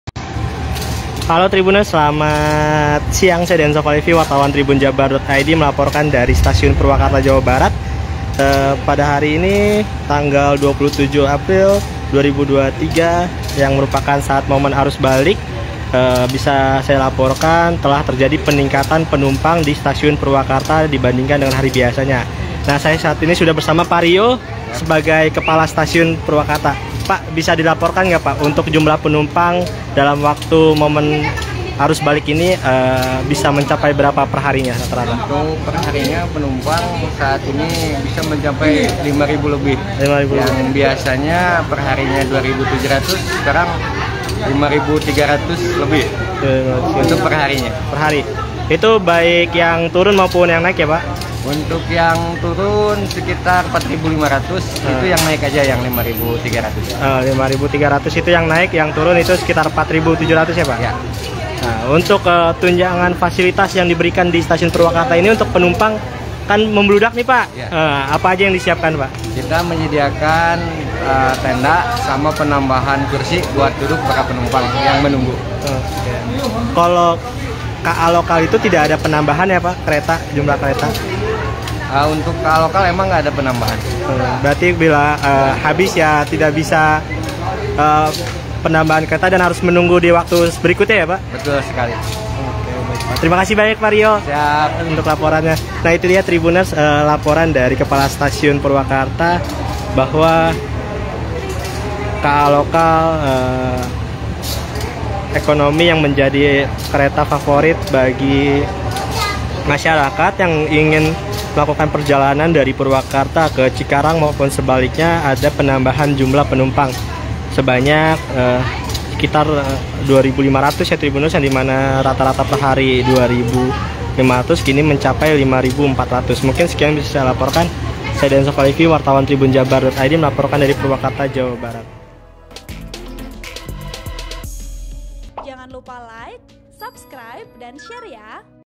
SEHARI STASIUN PURWAKARTA BISA LAYANI HINGGA 9.000 PENUMPANG MUDIK DAN ARUS BALIK LEBARAN 2023
Artikel ini telah tayang di TribunJabar.id dengan judul Sehari Stasiun Purwakarta Bisa Layani hingga 9.000 Penumpang Mudik dan Arus Balik Lebaran 2023,
Penulis: Deanza Falevi | Editor: Januar Pribadi Hamel|Video Editor: Uben